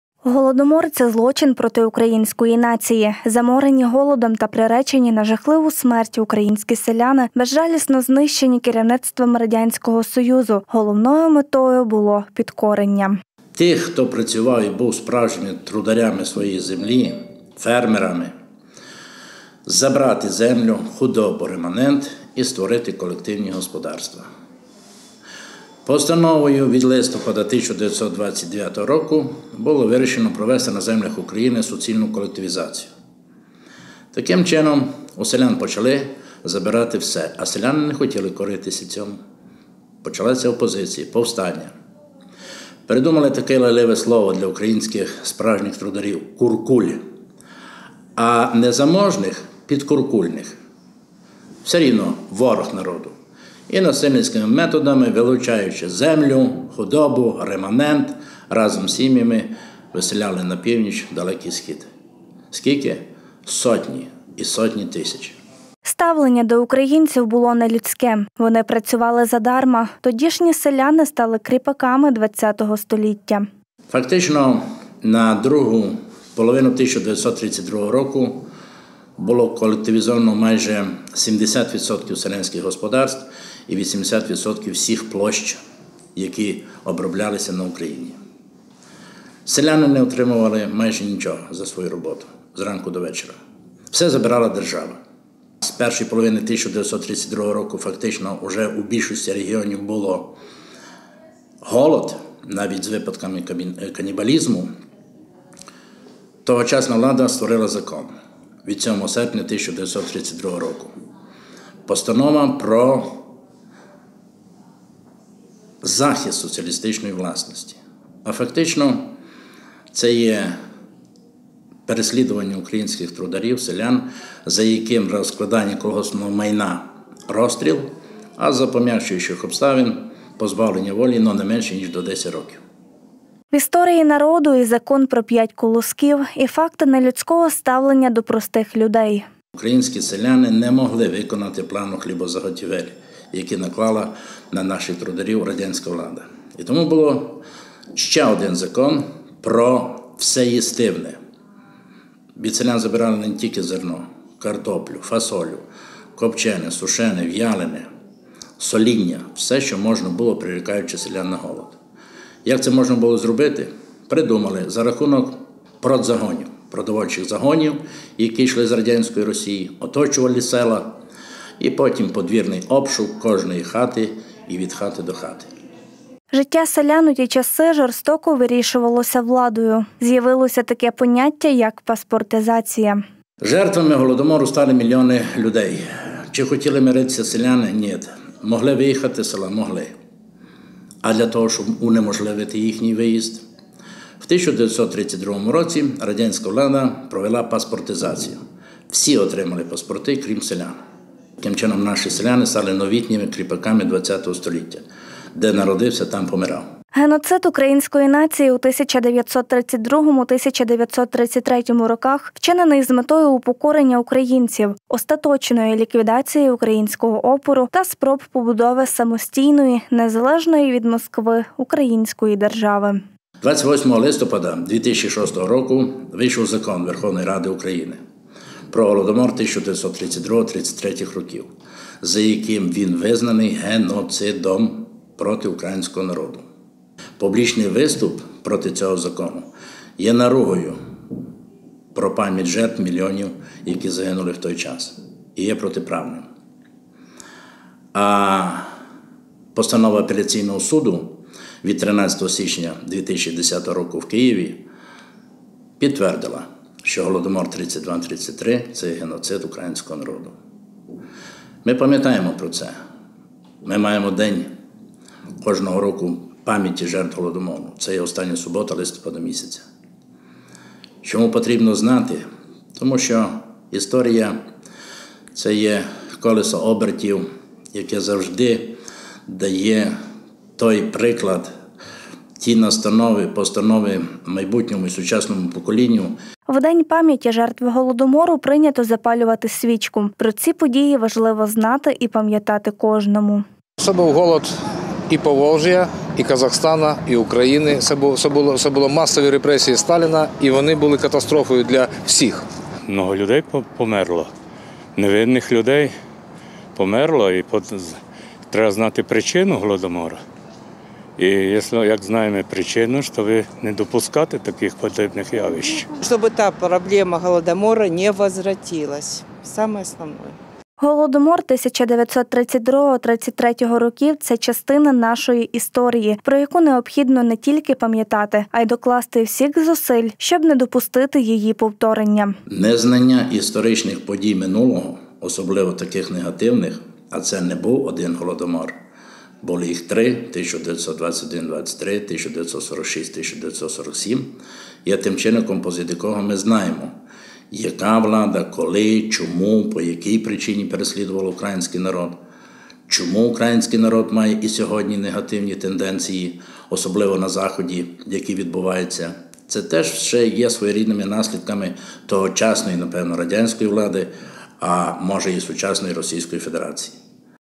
День пам’яті жертв Голодомору
Щорічно 27 листопада в Україні вшановують пам’ять жертв голодомору. Тих українців, що потерпіли від геноциду з боку Радянської влади. Близько 8 млн селян заморені голодом та приречені на смерть доживали віку без шматка хліба  у роті. Ці моторошні часи  до цих пір не залишають байдужими українську націю. Про ці події довгий час мовчали, проте зараз про них потрібно кричати та ніколи не забувати. Голодомор – це злочин проти української нації. Заморені голодом та приречені на жахливу смерть українські селяни безжалісно знищенні керівництвом Радянського Союзу. Головною метою – було підкорення. Ставлення до українців було нелюдське. Вони працювали задарма. Тодішні селяни стали кріпаками 20 століття. В історії народу і закон про 5 колосків і факти нелюдського ставлення до простих людей. Життя селян в ті часи жорстоко вирішувалося владою. З’явилось таке поняття як - паспортизація. Геноцид української нації у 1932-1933 роках, вчинений з метою упокорення українців, остаточної ліквідації українського опору та спроб побудови самостійної, незалежної від Москви Української Держави. В день пам’яті жертв голодомору прийнято запалювати свічку. Про ці події важливо знати і пам’ятати кожному. Голодомор 1932-33 років - це частина нашої історії, про яку необхідно не тільки пам’ятати, а й докласти всіх зусиль, щоб не допустити її повторення.